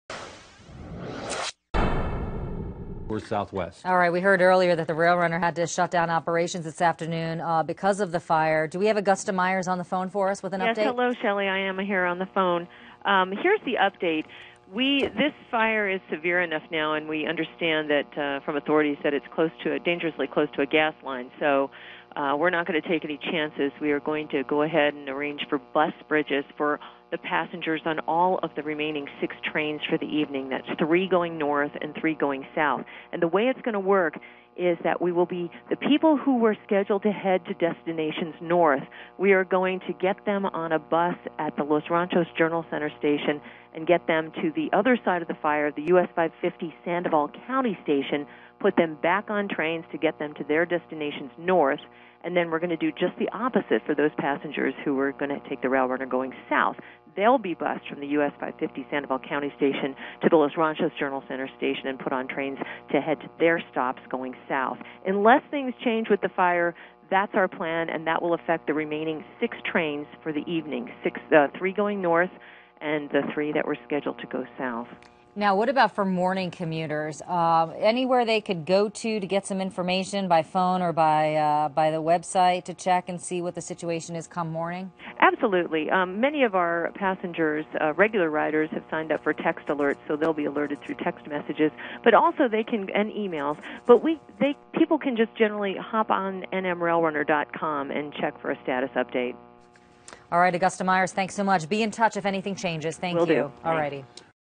Rail Runner sets up bus bridges after fire
The Romero Fire in the Corrales Bosque continues to burn actively on its east front near the Sandia Pueblo.